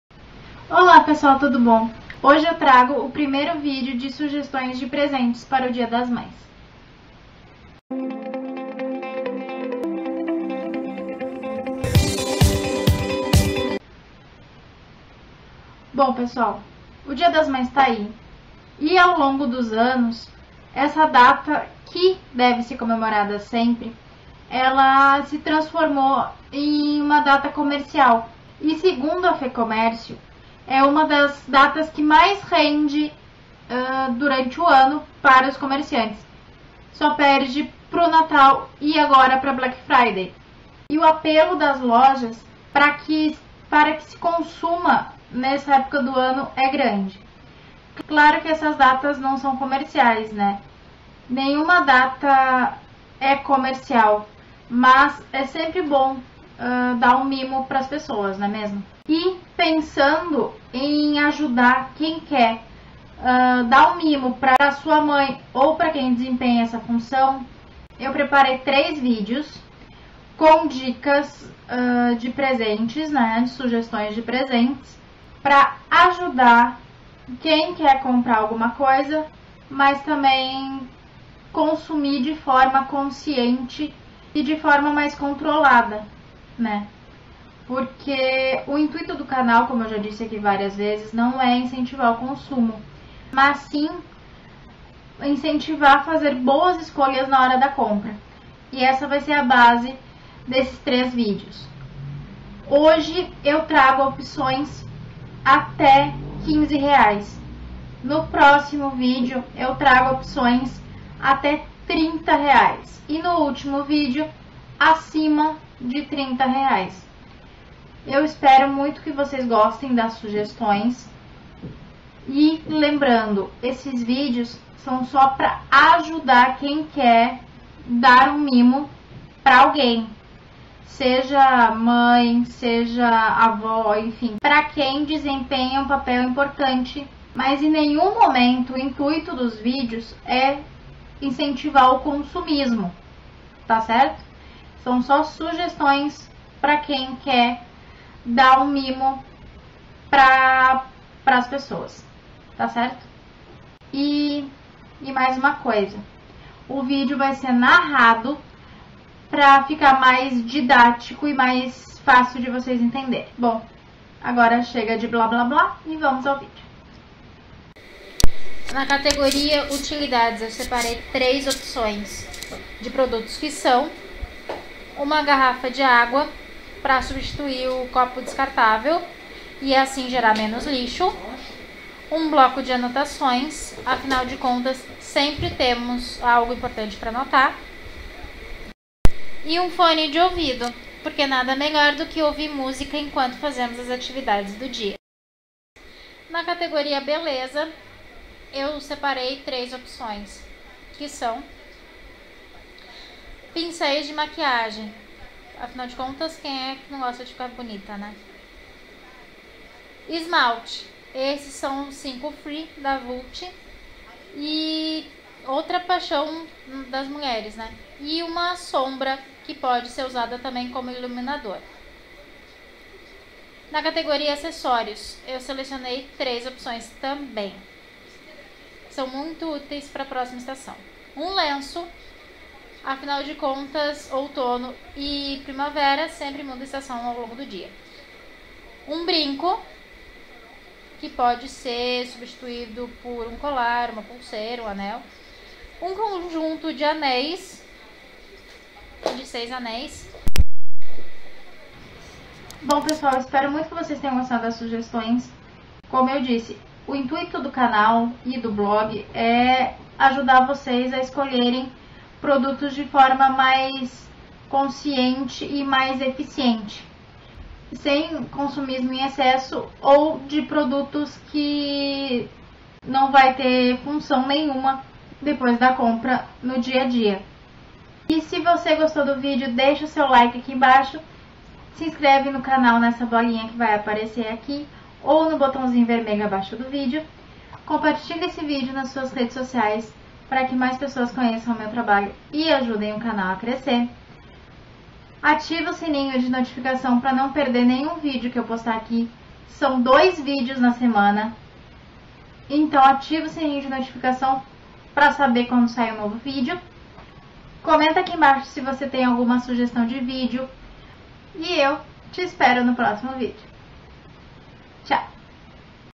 Sugestões de Presentes para as Mães_2018: EP1 Sugestões de Até de R$15,00
Olá pessoal,
O dia das mães está chegando e mesmo não sendo uma data comercial, é interessante dar um mimo para quem desempenha uma função importante na nossa vida, não é mesmo?!
Para te ajudar a encontrar um mimo funcional, original e que vá agradar, eu preparei uma série de três vídeos e nesse primeiro video apresento opções de mimos com valor até R$ 15,00.